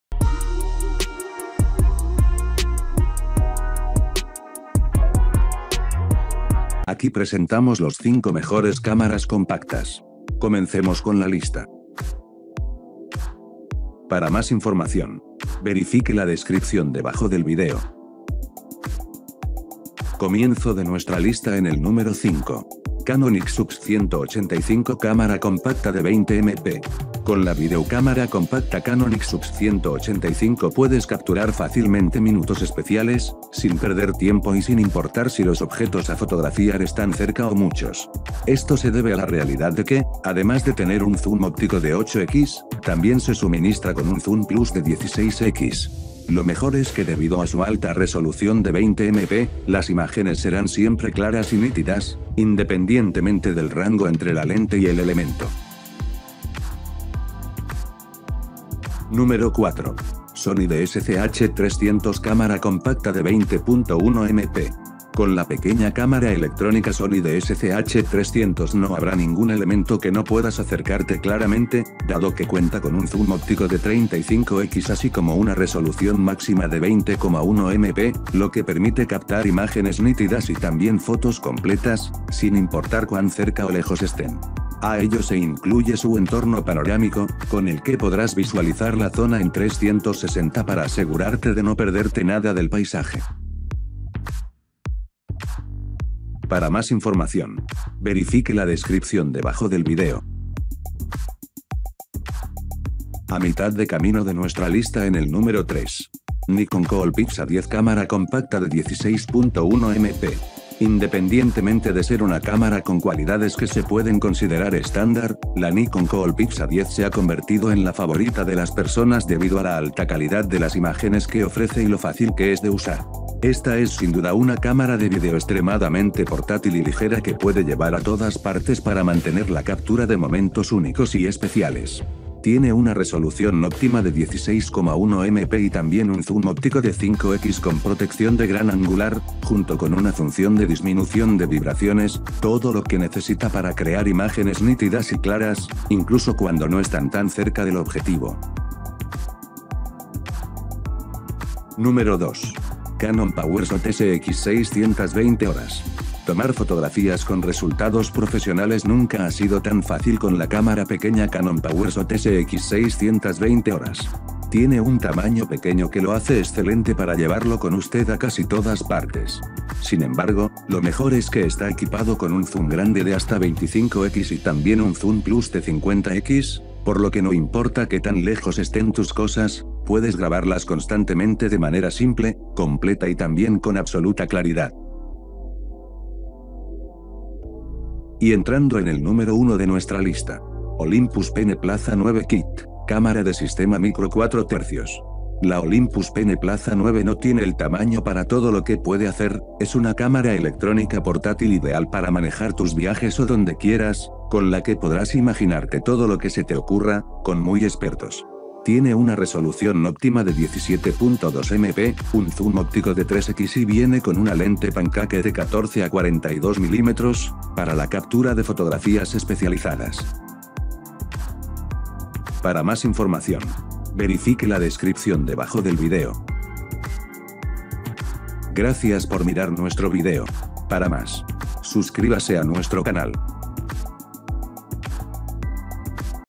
Los 5 Mejores Cámaras Compactas
Aquí está la lista de Los 5 Mejores Cámaras Compactas.

01. Olympus PEN E-PL9 Kit, Cámara de Sistema Micro Cuatro Tercios

02. Canon PowerShot SX620 HS

03. Nikon COOLPIX A10 Cámara compacta de 16.1MP

04. Sony DSC-H300 - Cámara compacta de 20.1 MP

05. Canon IXUS 185 - Cámara compacta de 20 MP